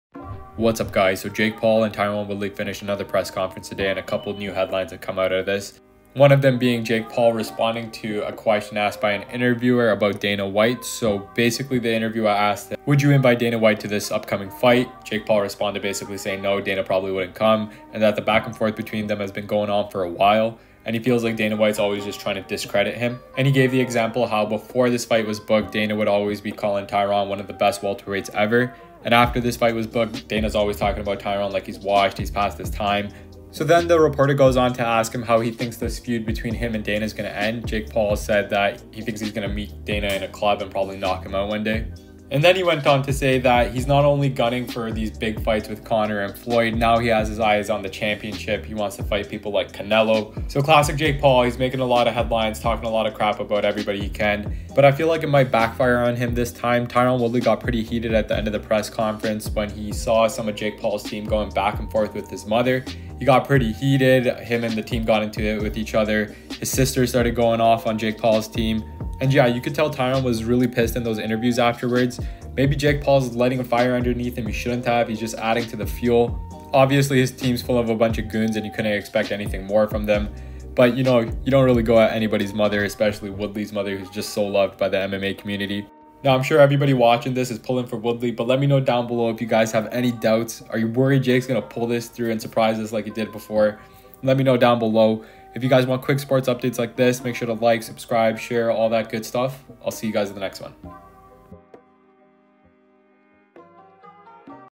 JAKE PAUL SAYS HE’LL KO DANA WHITE! Tyron Woodley Heated About Back and Forth With His Mother!
JAKE PAUL SAYS HE’LL KO DANA WHITE! Tyron Woodley Heated About Back and Forth With His Mother and Sister!